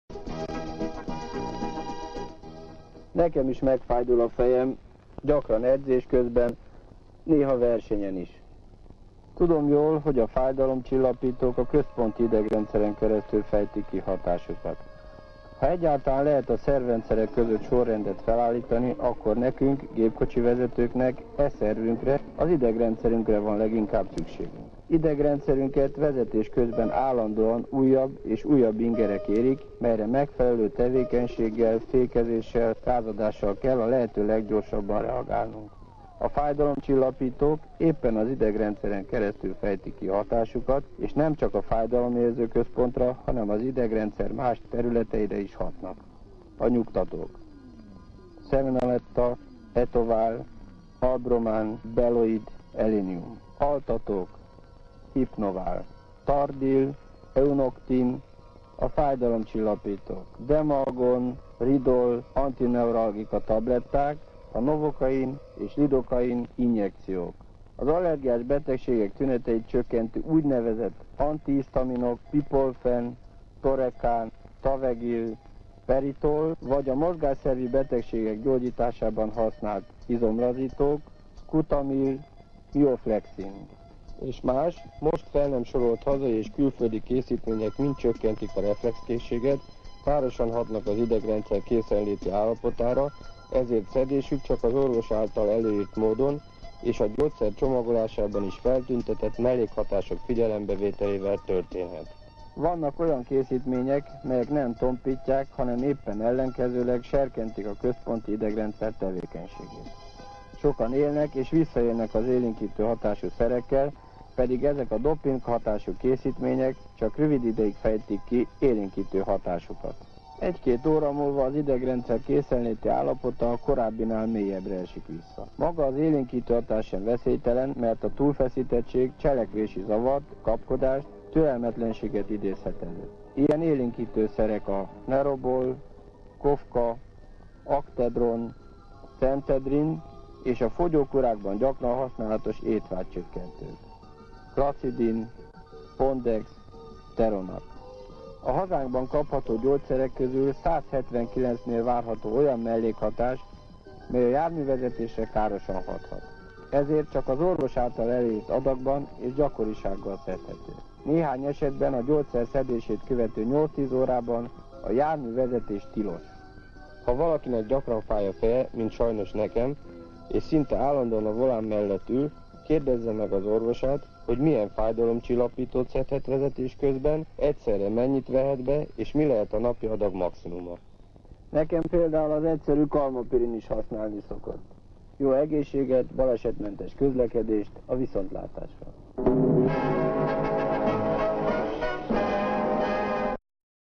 Gyógyszerfogyasztás és gépjárművezetés
Ferjáncz Attila jótanácsai 1985-ből